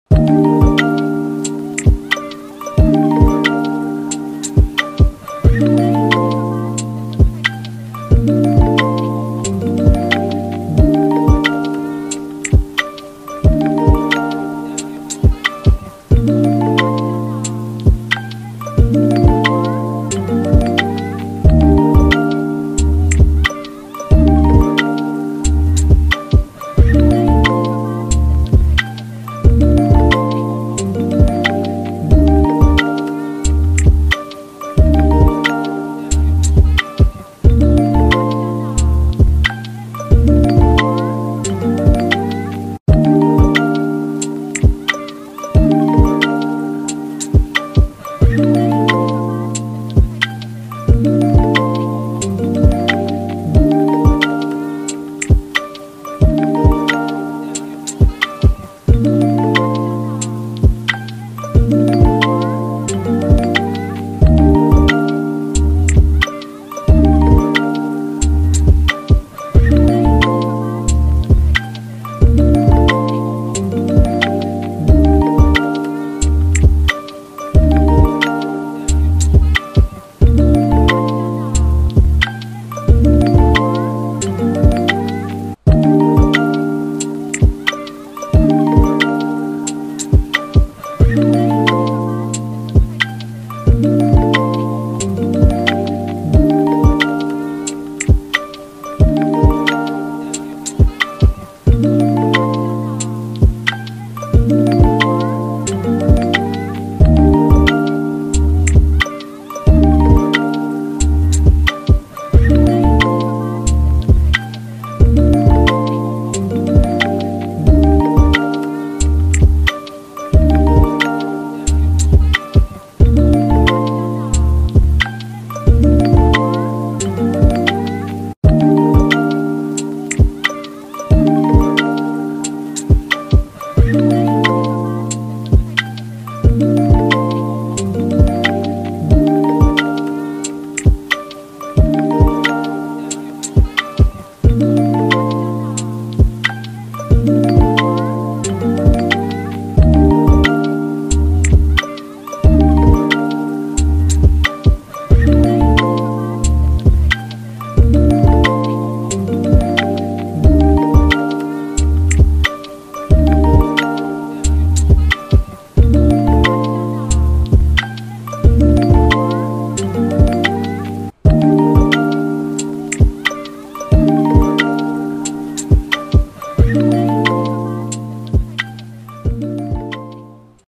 Bazelii and new creature!!! Creatures of Sonaria news || Roblox
The new robux creature would be obtainable in this weeks update with 1800R in robux gahca shop. At the same time, we have a new creature, it would be going to the galaxy explorer gacha and would be obtainable with 500 mush and a galaxy explorer token!!!